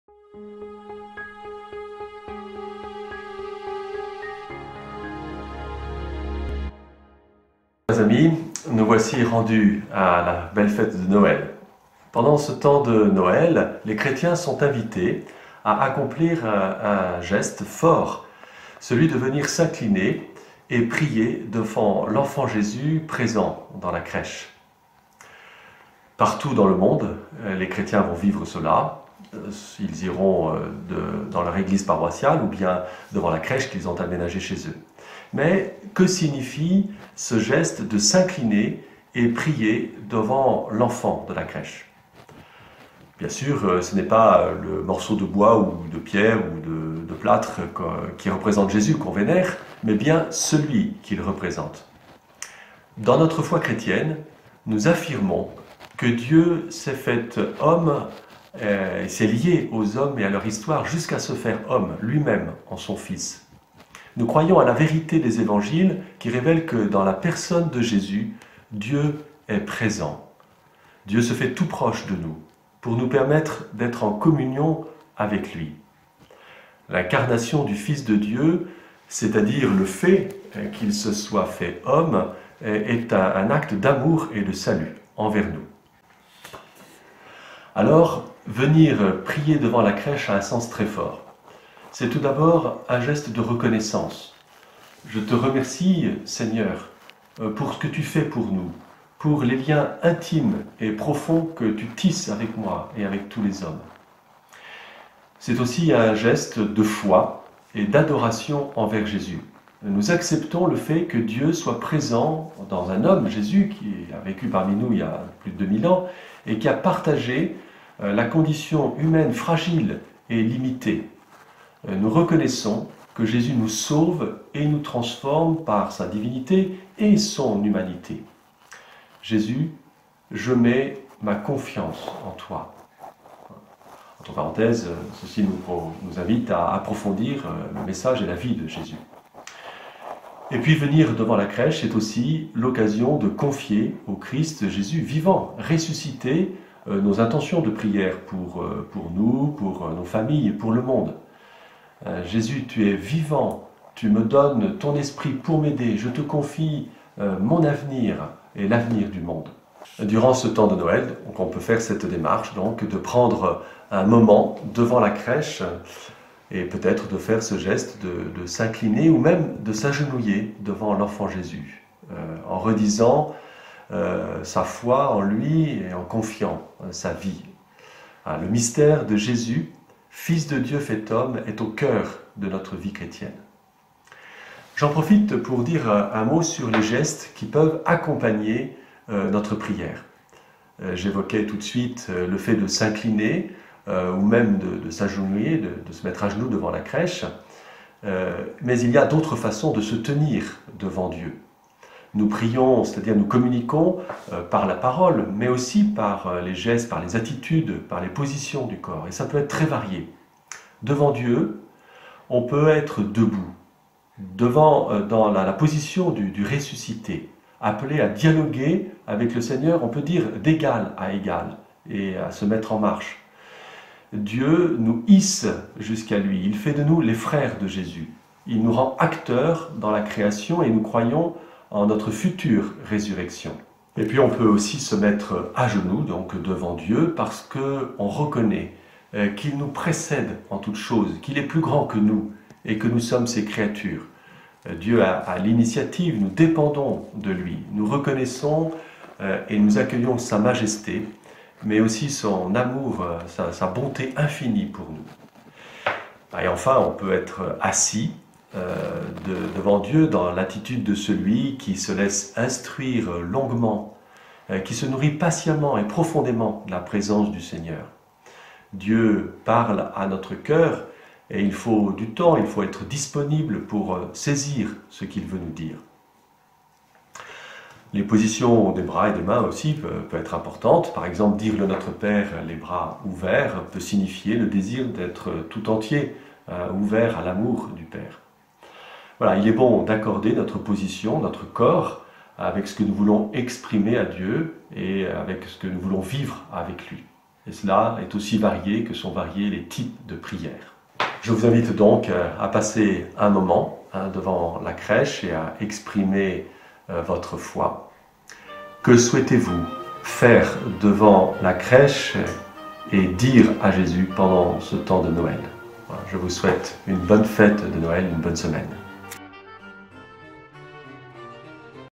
[Tuto Prière Catho] 5- Prier devant la crèche
Tutoriel proposé par le Père Bruno Guicheteau, prêtre de l'église catholique en France, curé de la paroisse Saint Etienne de Grandmont (diocèse de Tours)

Réagissez à ce tuto sur la page facebook de la paroisse Saint Etienne de Grandmont